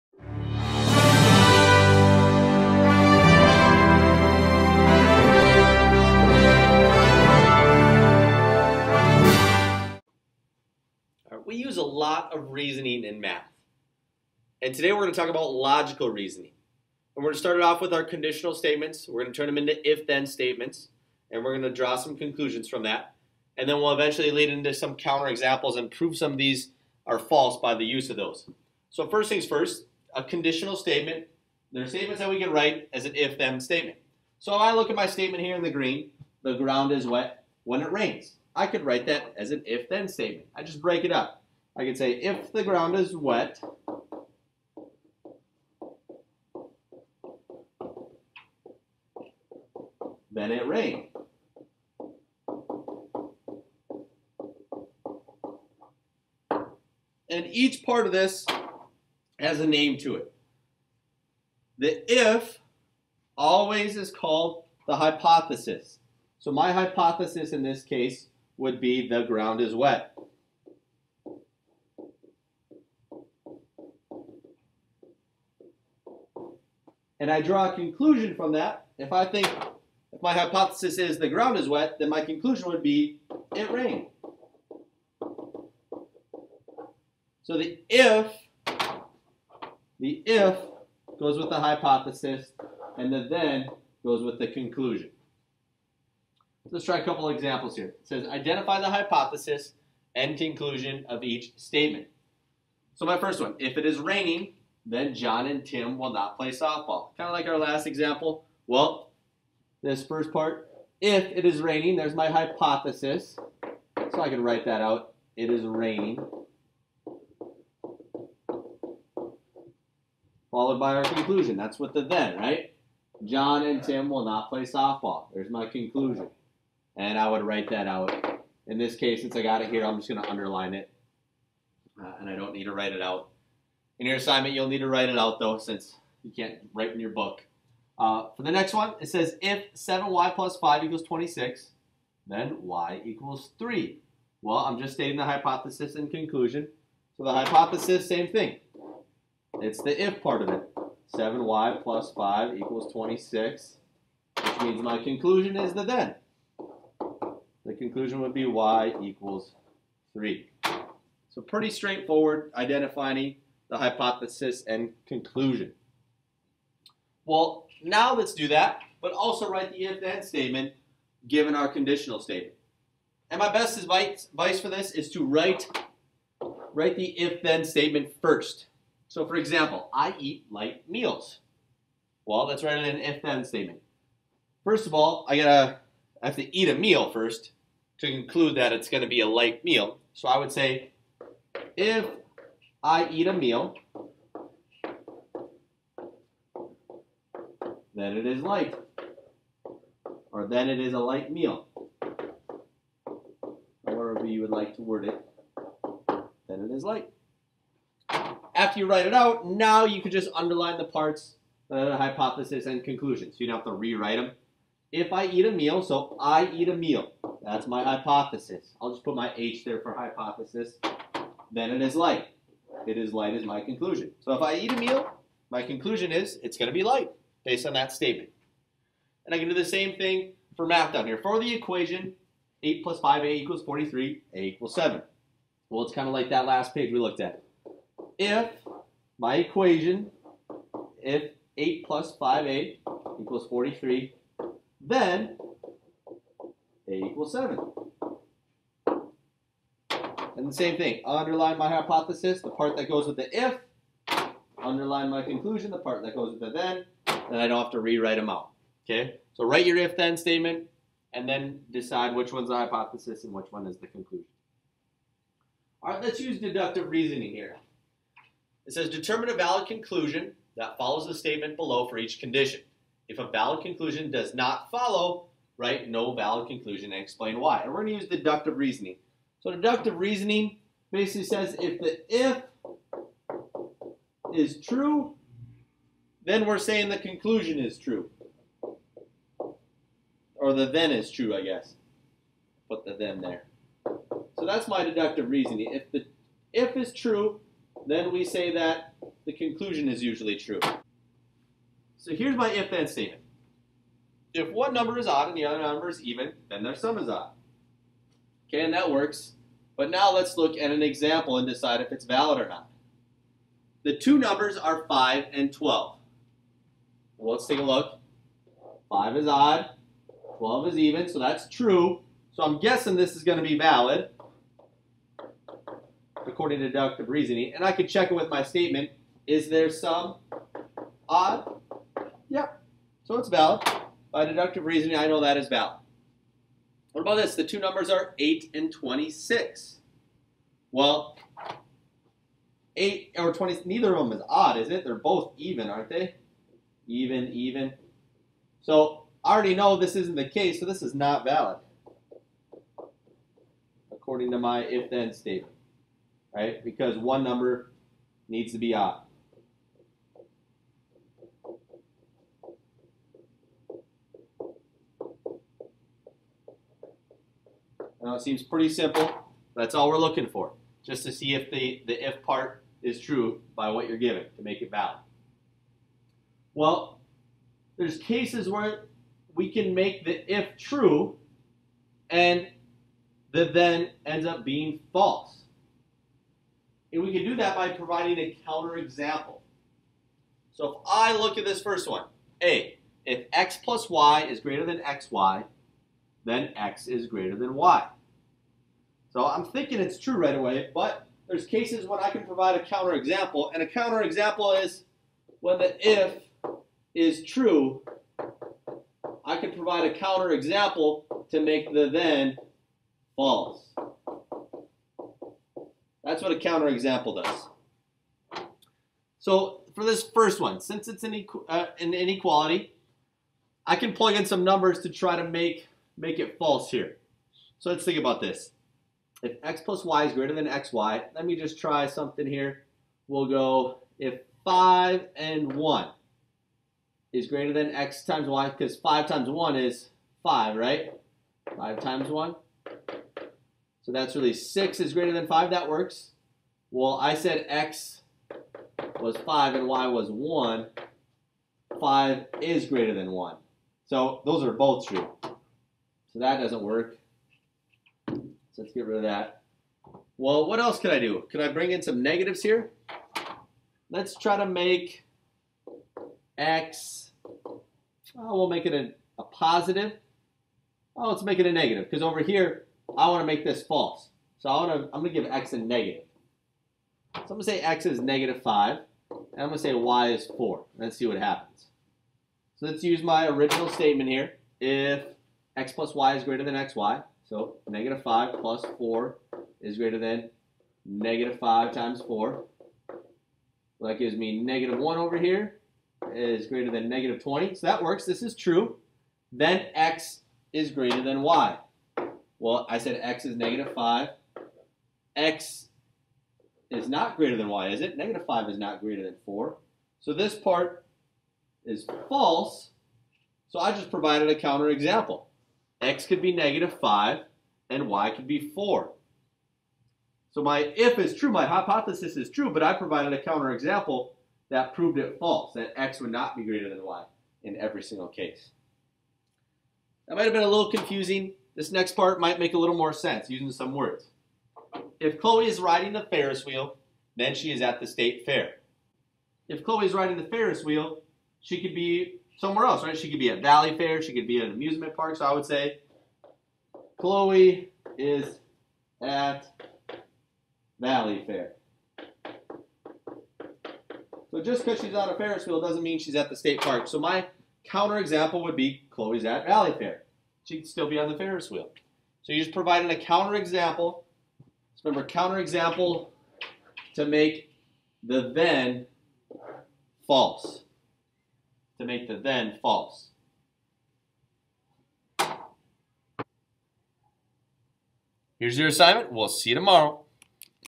Algebra I - Logical Reasoning & Counterexamples
Algebra I lesson over Logical Reasoning & Counterexamples